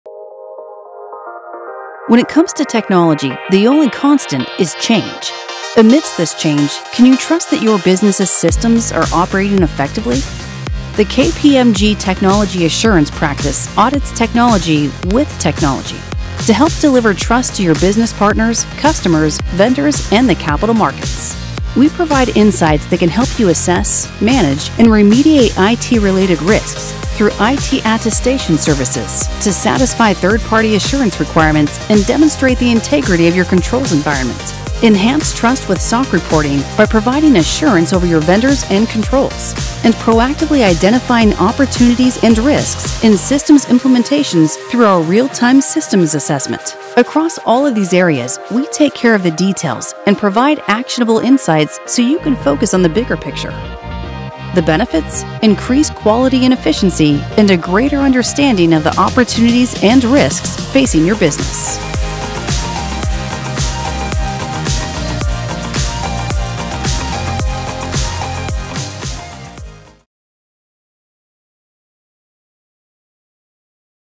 KPMG Technology Assurance
KPMG Technology Assurance audits technology with technology to help assess, manage and remediate IT related risks. Learn how assuring your technology can help improve quality and efficiency in your business.